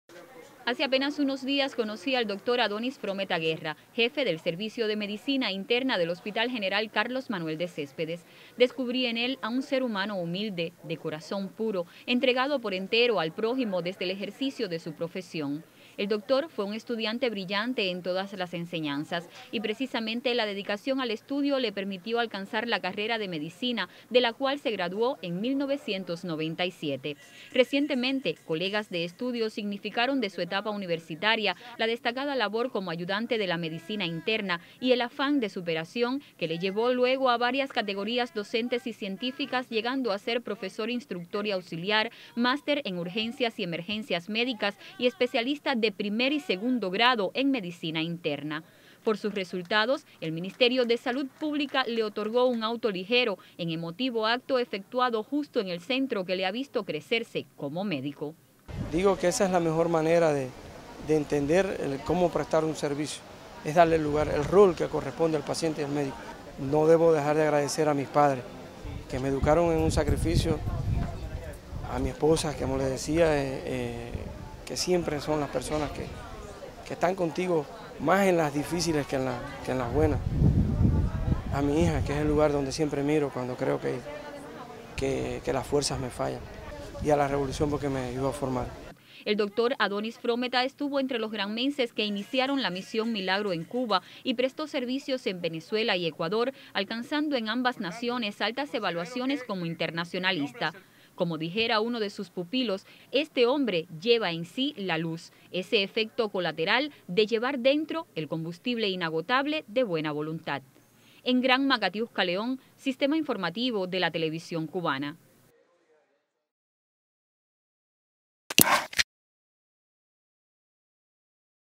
Reconocen a médico granmense por su destacada trayectoria
El doctor Adonis Frómeta Guerra, Jefe del servicio de Medicina Interna del Hospital Carlos Manuel de Céspedes y presidente de la sociedad provincial de la especialidad en Granma, ha destacado por sus resultados académicos, científicos y la asistencia al paciente. Por los méritos fue reconocido por el Ministerio de Salud Pública en Cuba.